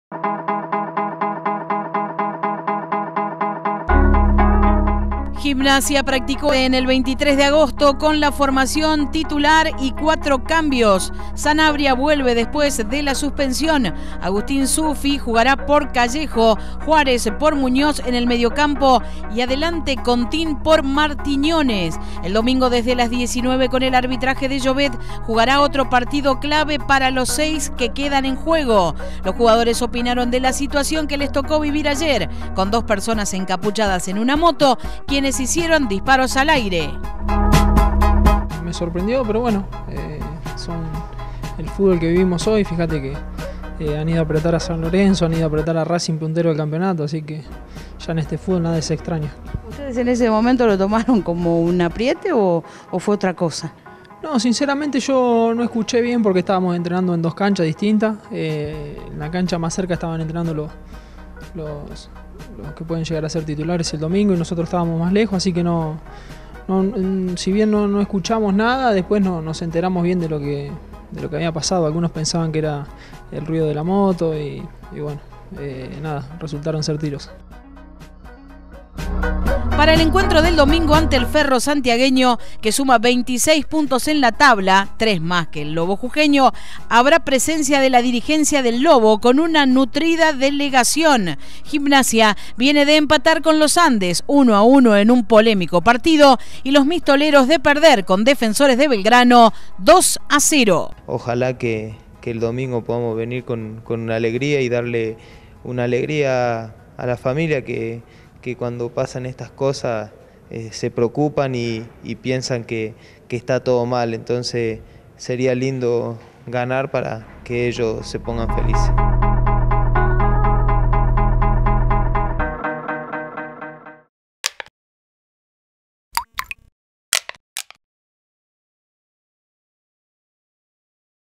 Después del apriete gimnasia hizo futbol
Semana intensa para el lobo, que luego del apriete de la barra, el plantel hizo fútbol. Jairo, paró un equipo para enfrentar a Central Córdoba.